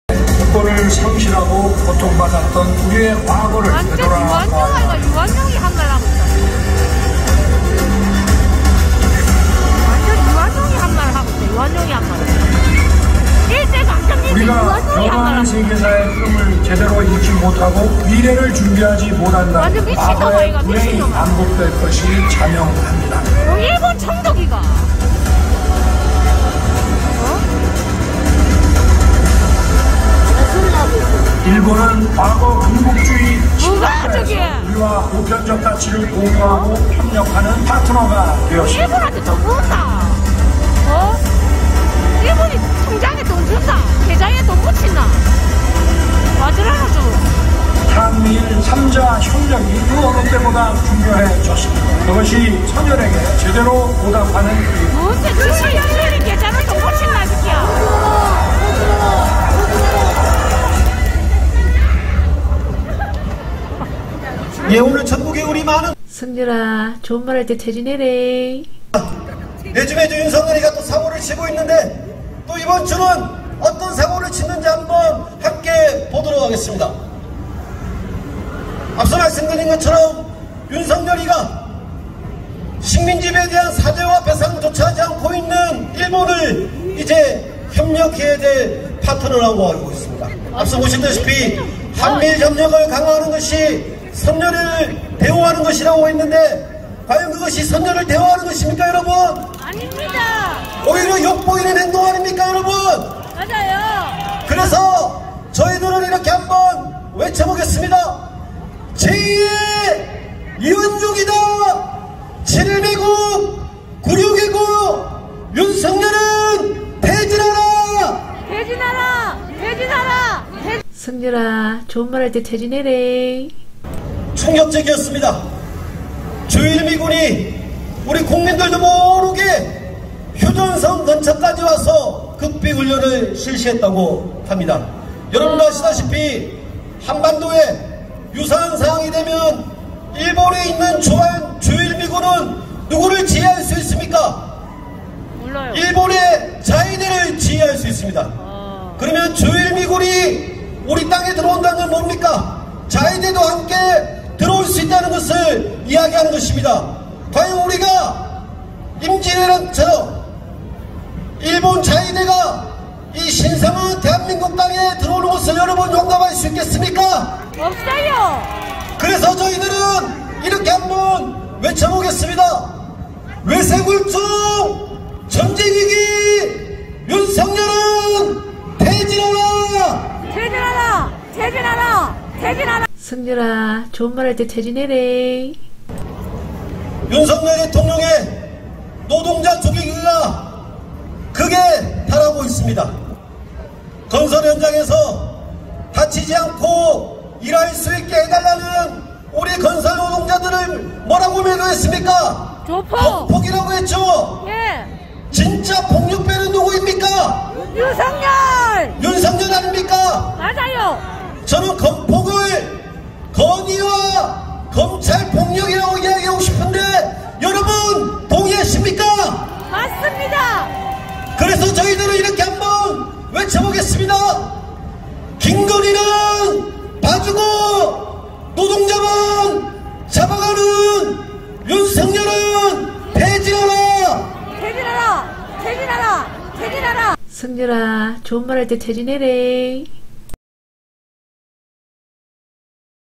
💙💙💙부산촛불집회, "윤폭아, 너의 매국에 한국인을 끌어들이지말고 너 혼자 일본협력파트너 해!"
I originate the Daedong world.